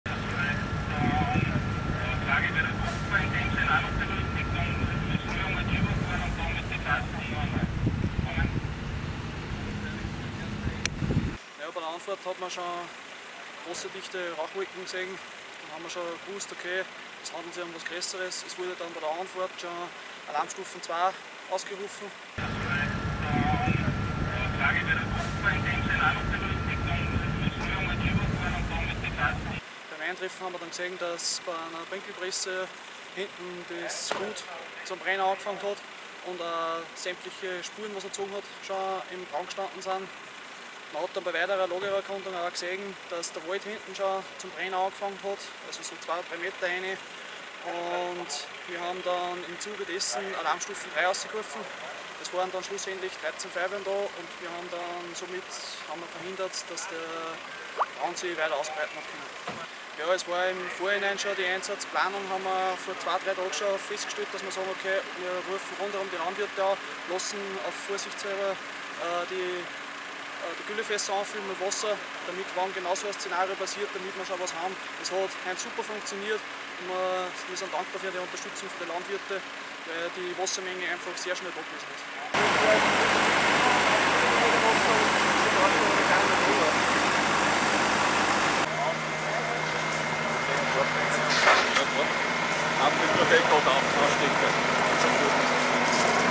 Alarmstufe 3 - Waldgebiet in Flammen: Quaderballenpresse löst Brandkatastrophe aus
Pregarten, 9. Juli 2023 - Ein Brand einer Quaderballenpresse auf einem Feld im Gemeindegebiet von Pregarten hat sich rasch auf den angrenzenden Wald ausgebreitet und stellt die örtlichen Feuerwehren vor eine enorme Herausforderung. Der Einsatz wurde gegen 14:30 Uhr ausgelöst und mittlerweile sind 13 Feuerwehren damit beschäftigt, den Brand unter Kontrolle zu bringen und erfolgreich zu löschen.

Das Feuer breitete sich aufgrund der trockenen Witterungsbedingungen und starken Winde schnell aus, was zu einer erheblichen Ausdehnung des Brandherdes führte. Die Feuerwehren aus den umliegenden Ortschaften eilten zur Unterstützung herbei und setzen alle verfügbaren Ressourcen ein, um die Flammen einzudämmen. Wir werden Sie über die Entwicklungen auf dem Laufenden halten.